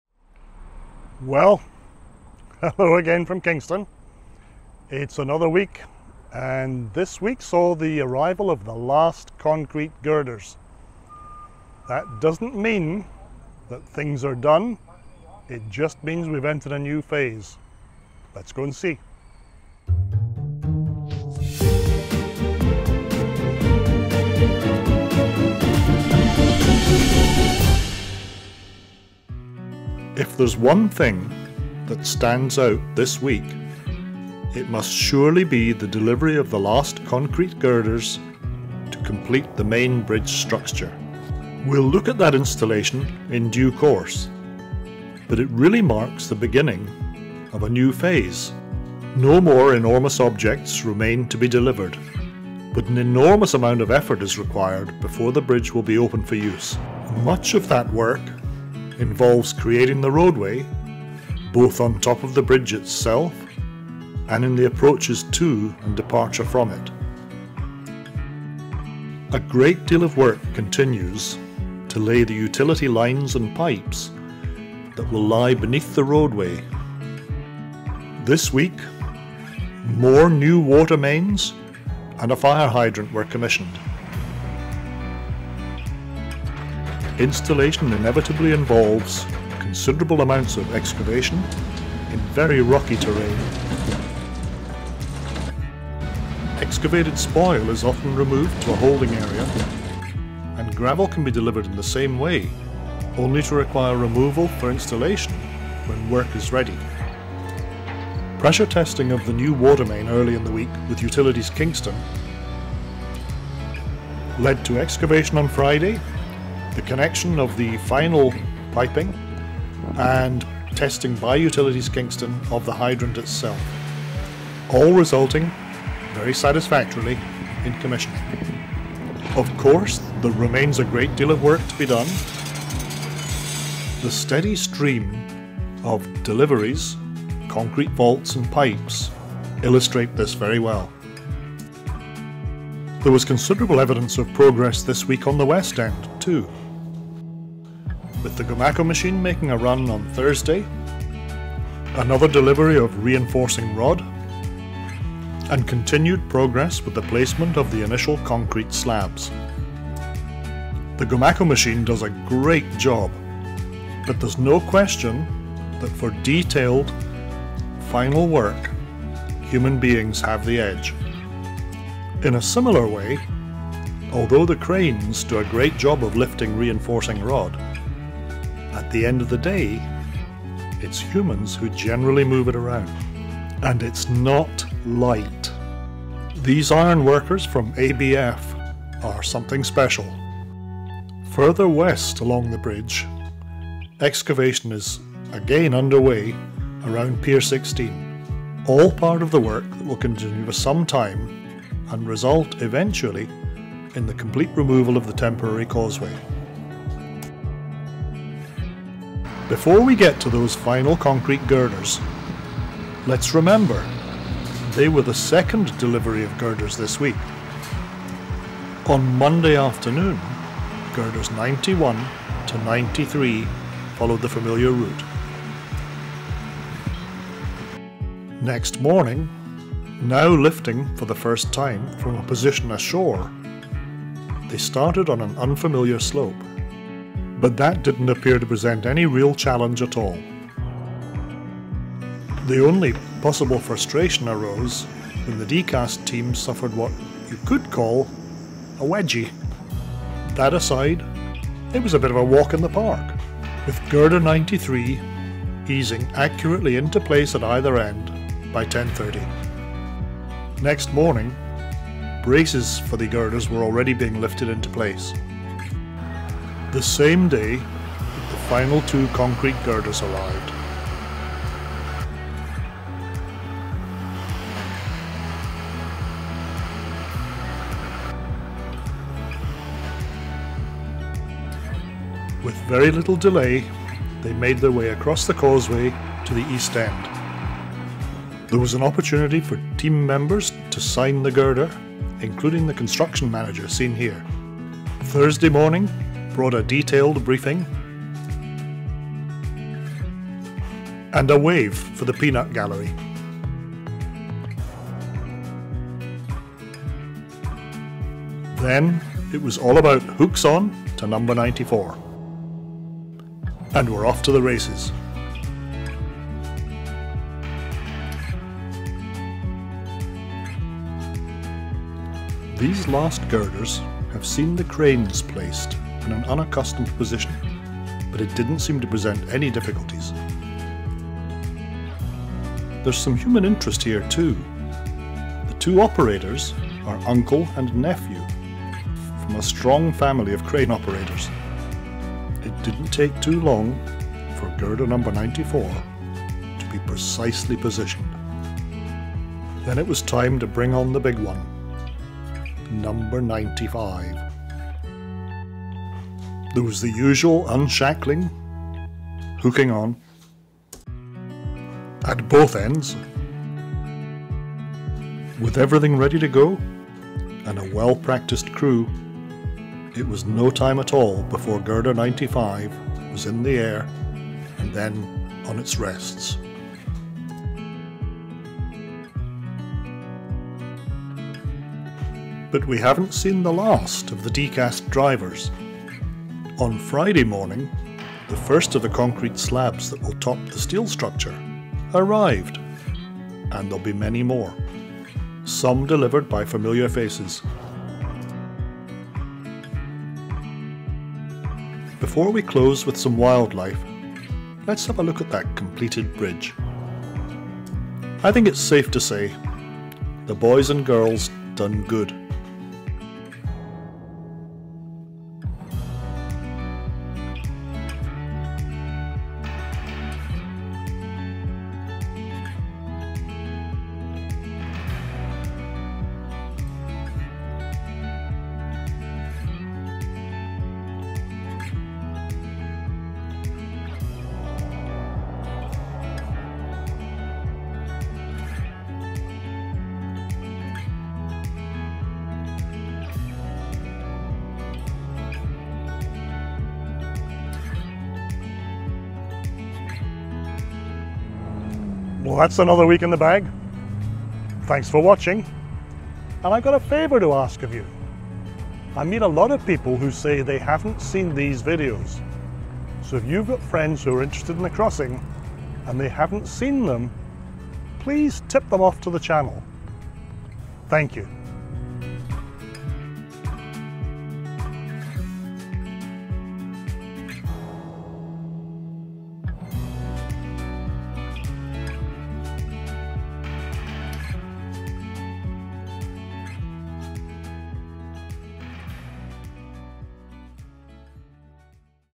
It's All Over Now 4K
With the arrival of the last concrete girders, job done on the Third Crossing - or is it ?

Chapter Markers:
00:00 - Introduction
00:29 - Overview of the Final Girder Installation
02:22 - Work on the West End
03:28 - Arrival of the last 5 concrete girders
04:25 - The Final Girders arrive
06:52 - Wildlife